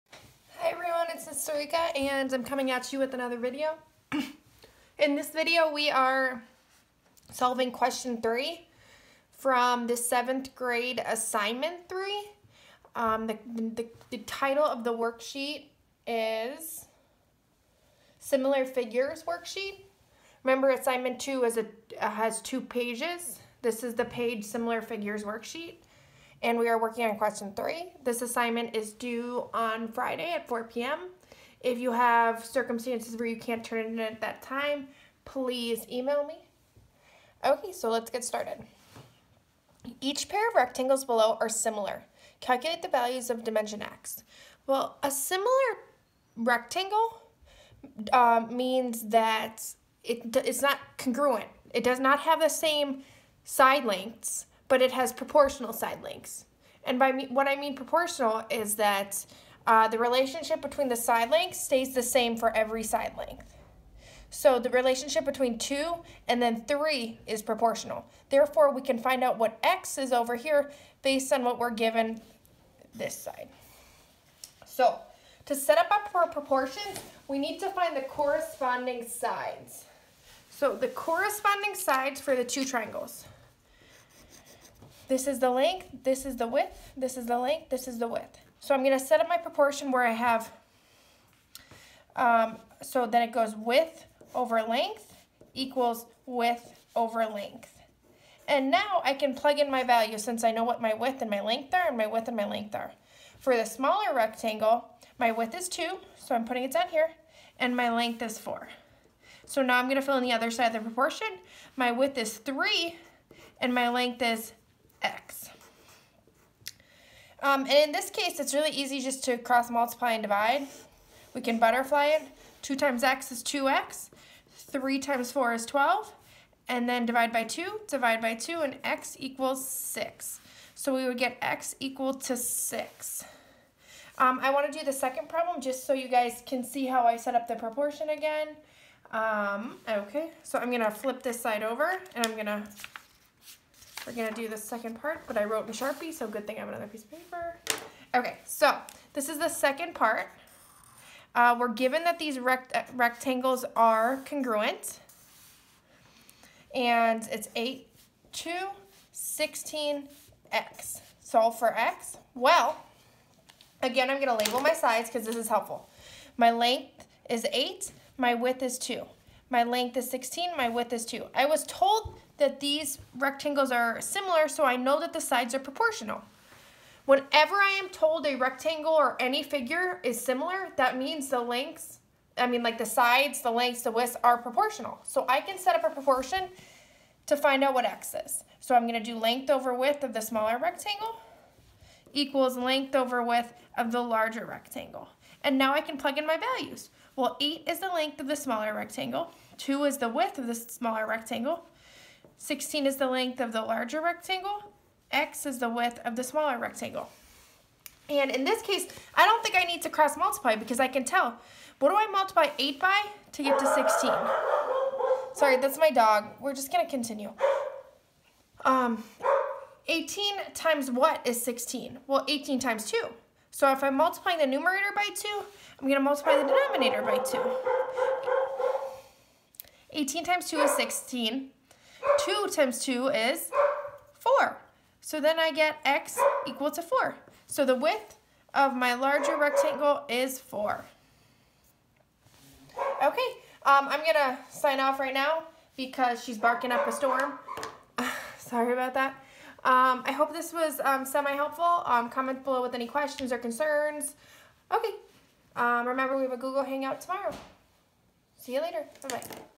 assignment 3 - similar figures worksheet - question #3
Hi,
In this video, I solve question 3 from the 7th grade assignment 3. I use proportions and the properties of similar figures to solve for x. I hope this is helpful.